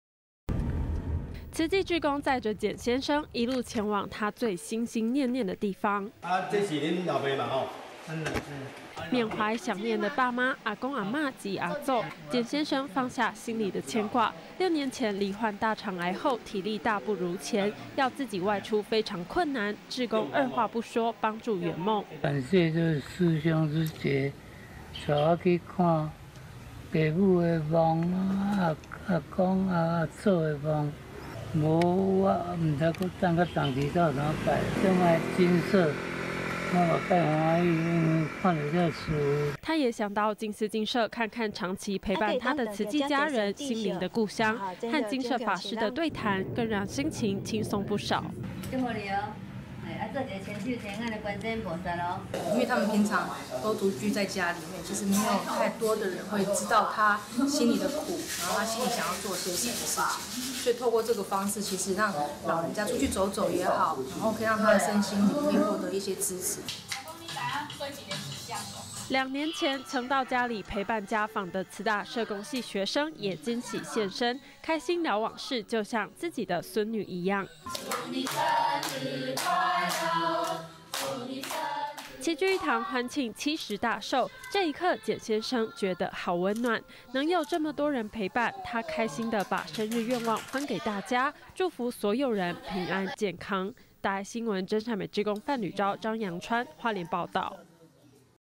想再看看父母長眠地 志工助癌友圓夢
一位簡先生，罹患大腸癌六年的簡先生，身體每況愈下。今年四月社工偕同慈濟志工訪視團隊，關懷簡先生，進一步為他圓滿生日願望。